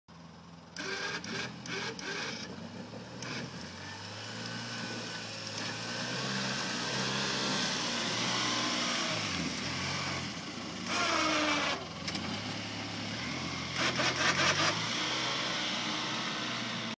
Polaris Ranger automatic front Yard scraper, avaiable from allspares atv, trade inquireis welcome